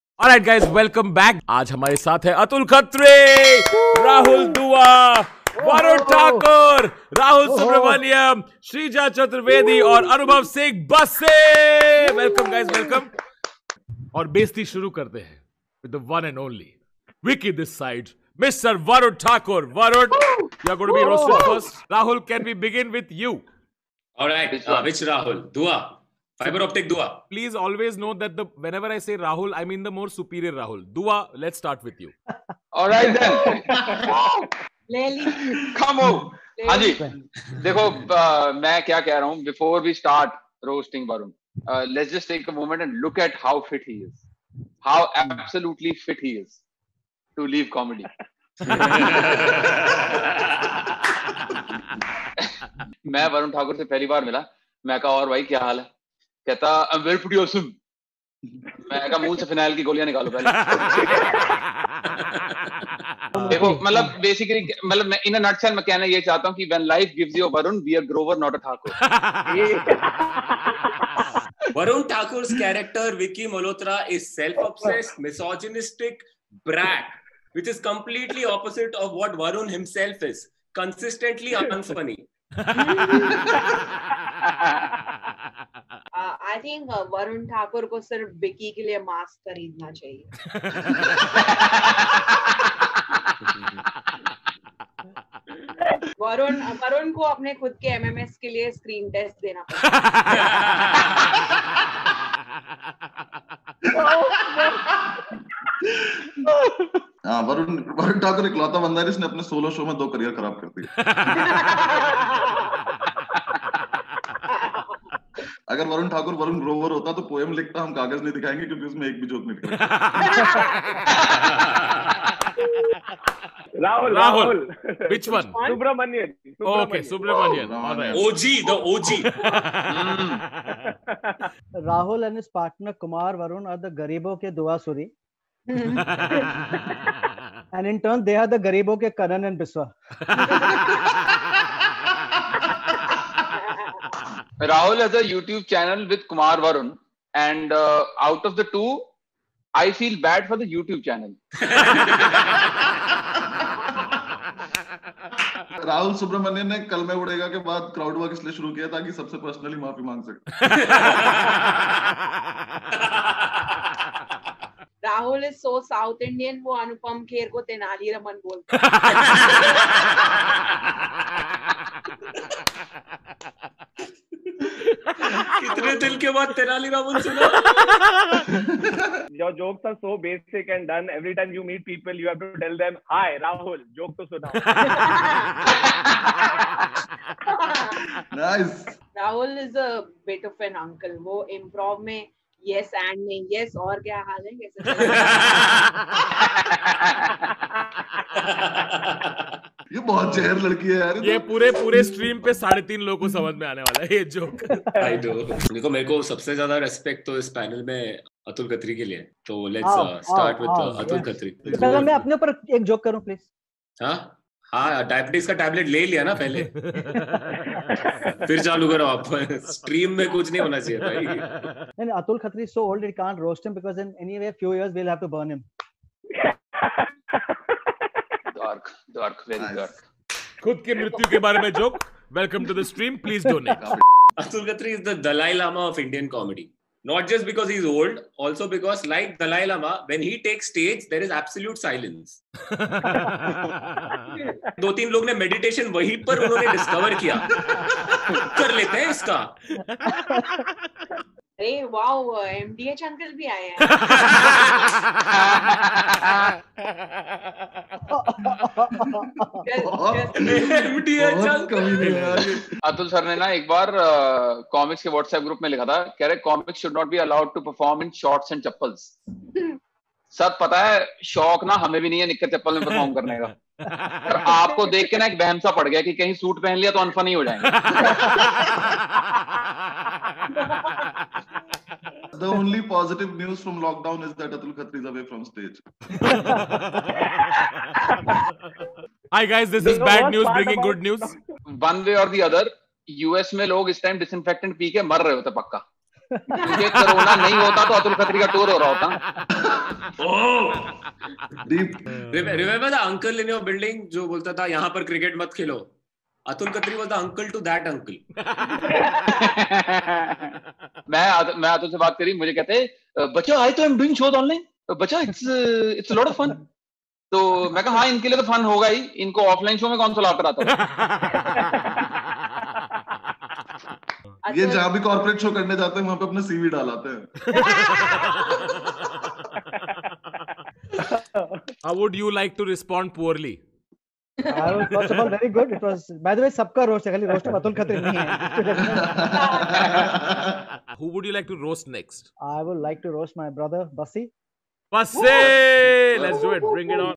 STAND UP COMEDIANS ROAST EACH OTHER FT. TANMAY BHATT | PART - 1
In this video, I’ll show you COMEDIANS ROAST EACH OTHER FT. TANMAY BHATT | PART - 1 Enjoy!